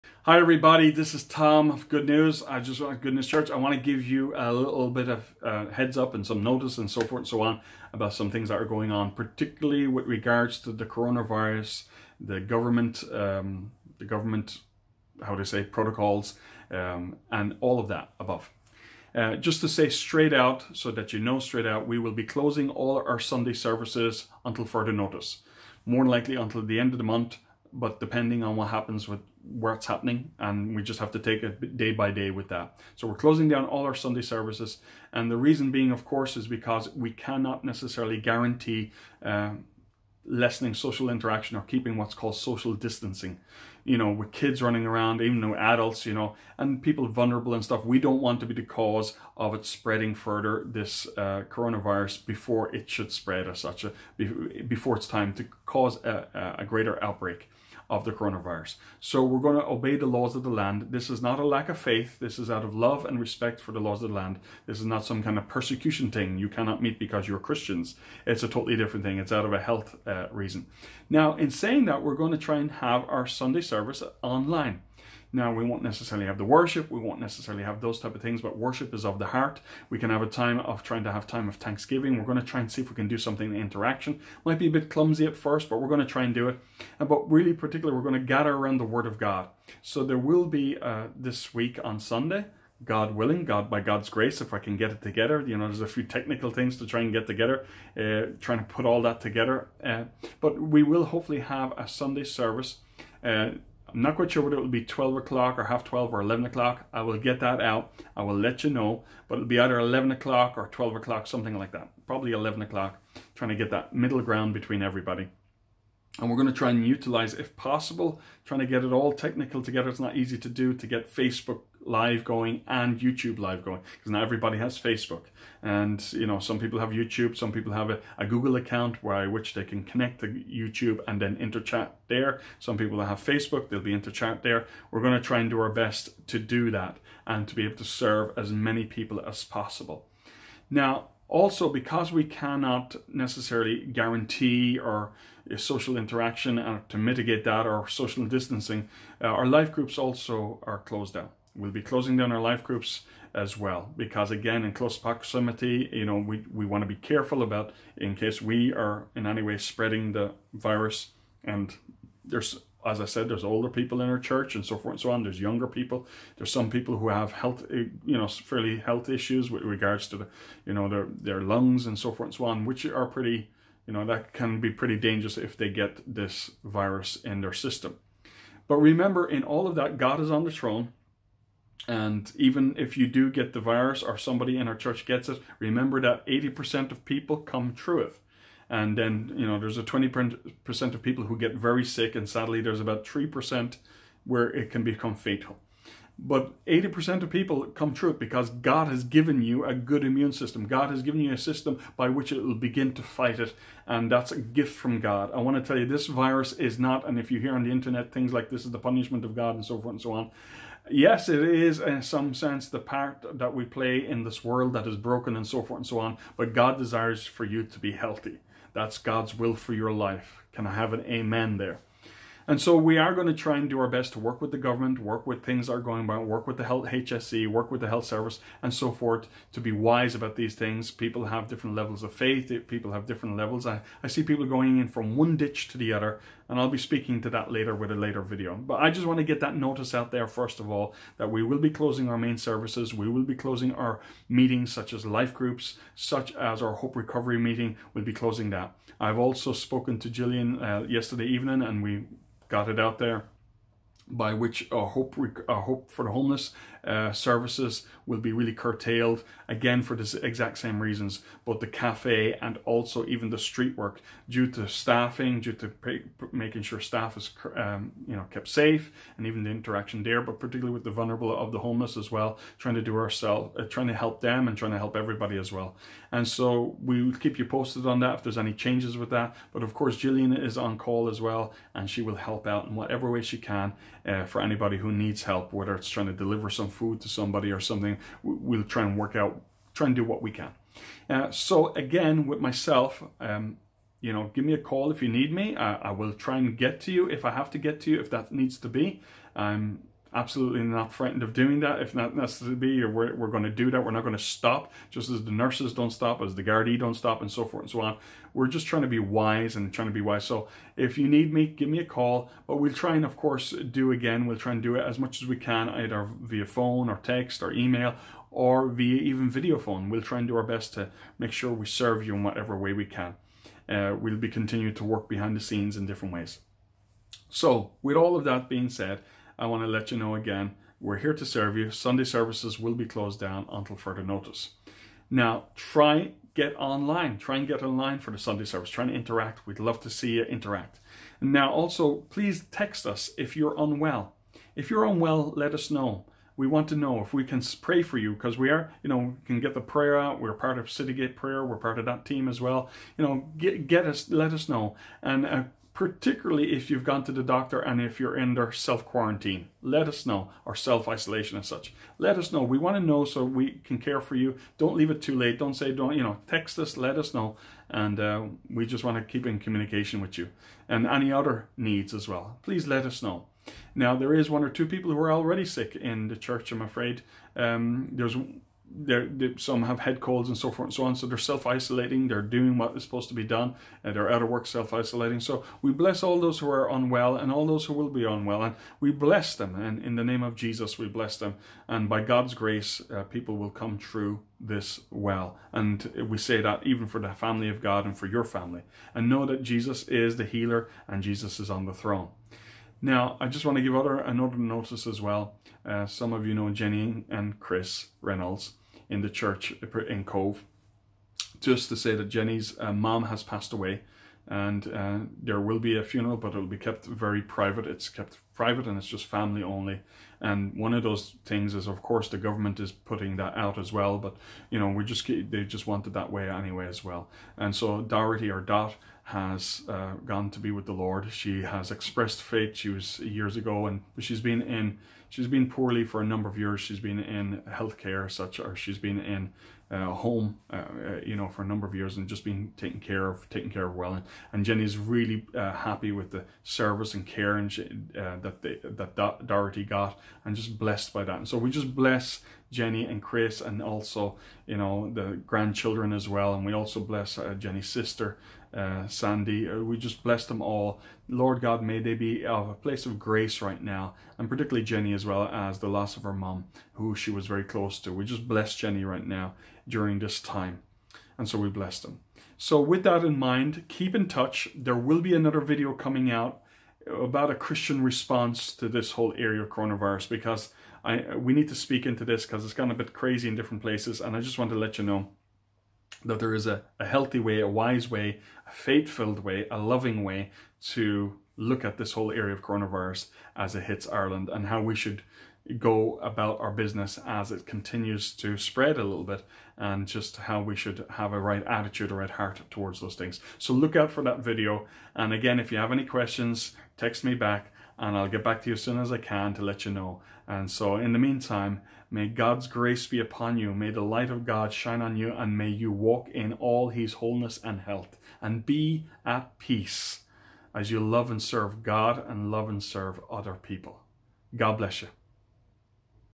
Good News Church Announcement - 13th March 2020
This is letting people know about the close of church services due to the coronavirus.